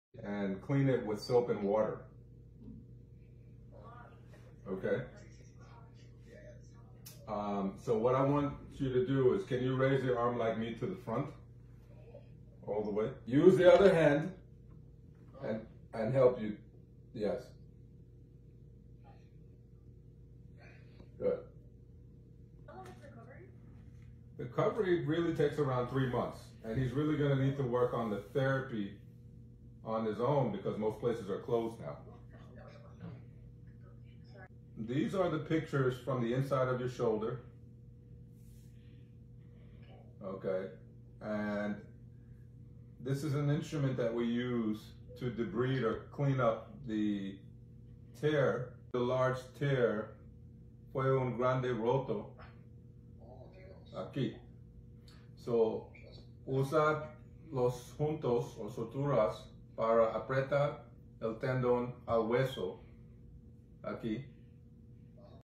Telemedicine: Patient Follow-Up
In efforts to keep with social distancing, we use Telemedicine to monitor progress and restore mobility. Here is a patient following up after arthroscopic shoulder surgery for a rotator cuff repair.